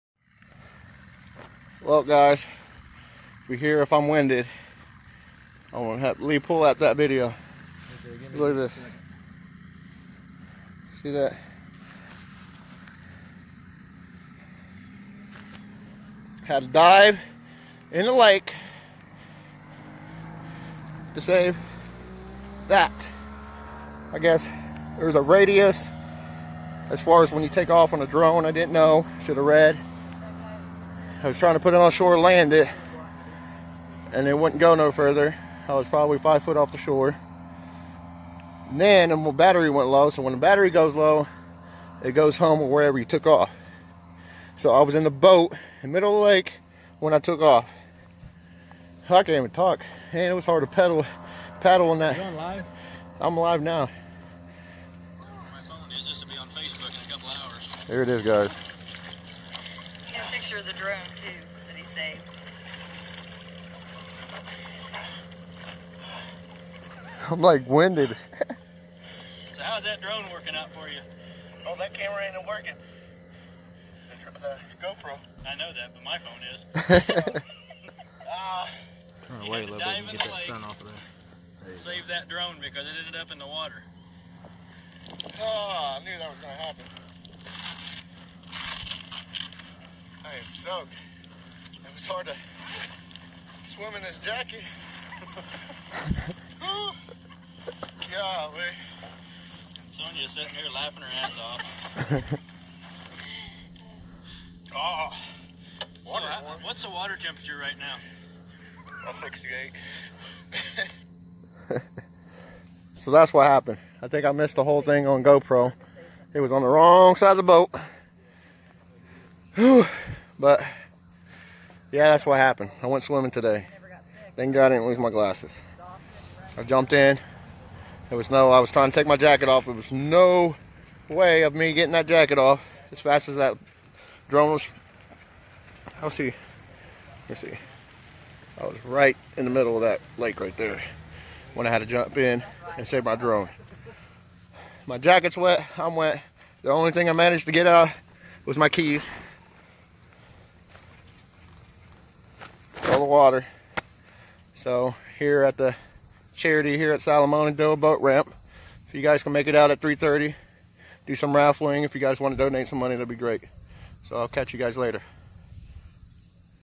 Almost lost my DRONE and went swimming! Summer of 2017
Went swimming for my drone in the middle of Salamonie Reservoir.